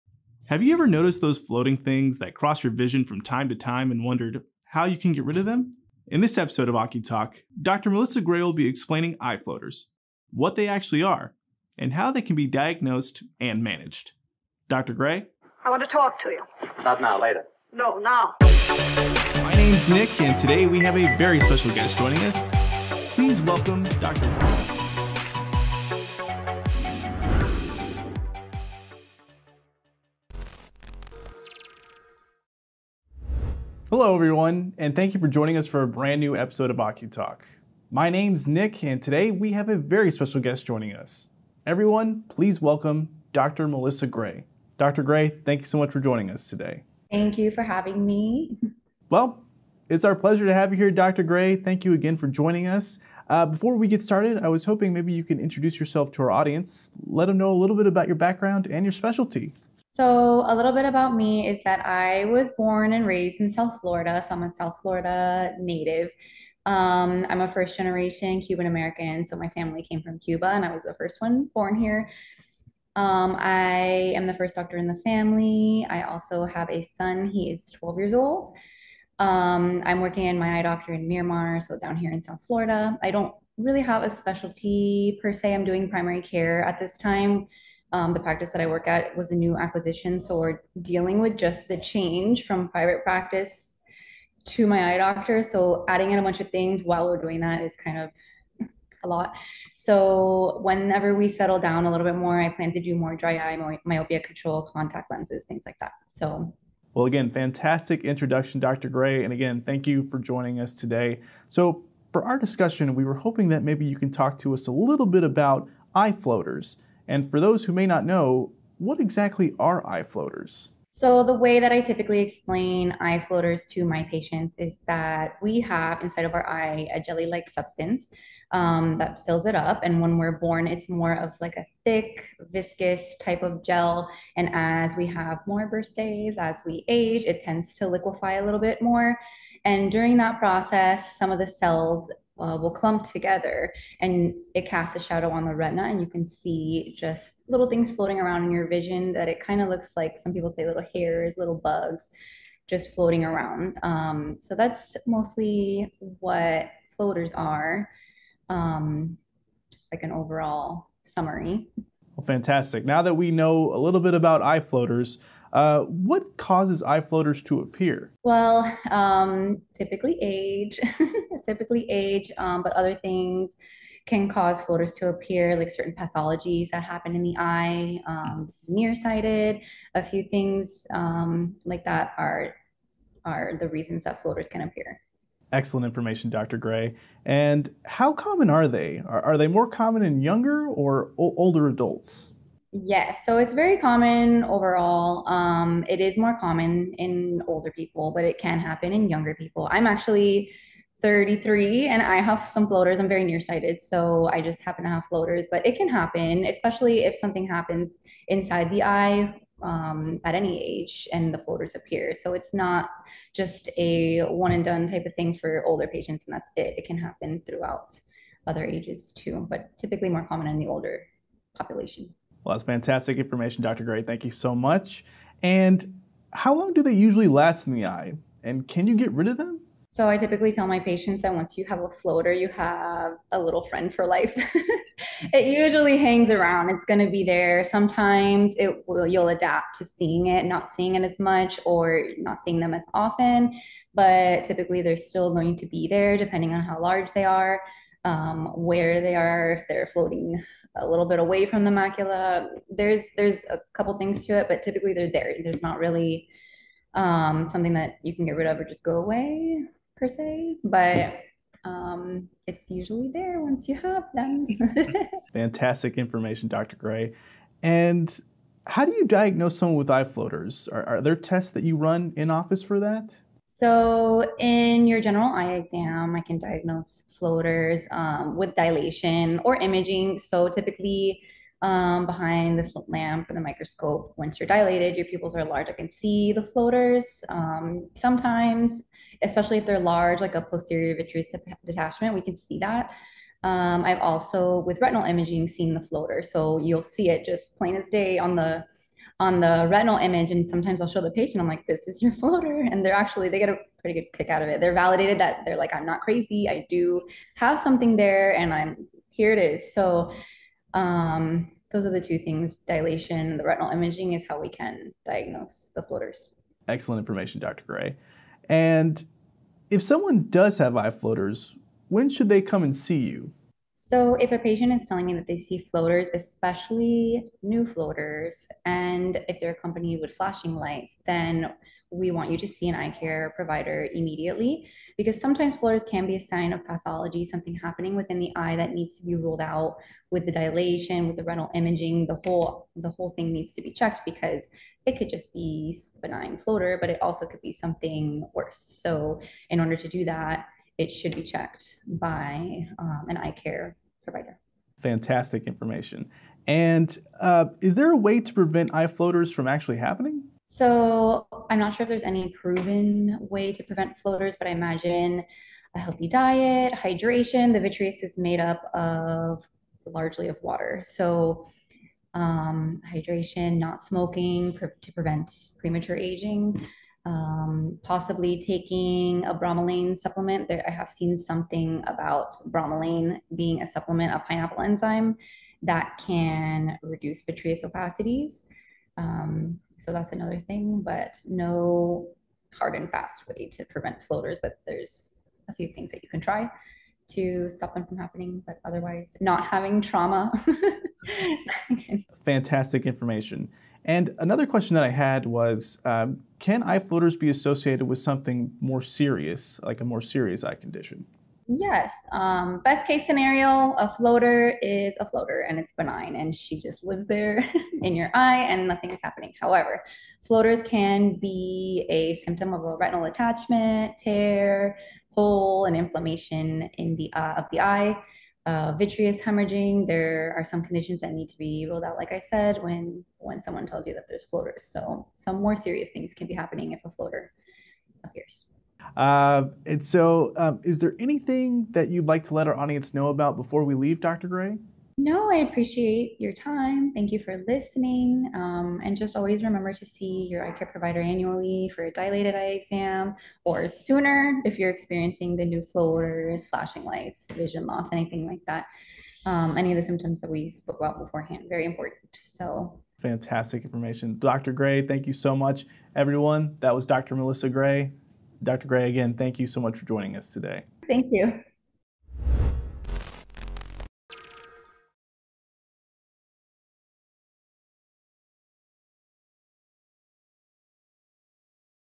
Everything You Should Know About Eye Floaters | Dr. Melissa Gray Explains
Have you ever noticed those floating things that cross your vision from time to time and wondered how you can get rid of them? In this episode of OCuTALK™, Melissa Gray, O.D. will be explaining Eye Floaters, what they actually are, and how they can be diagnosed and managed.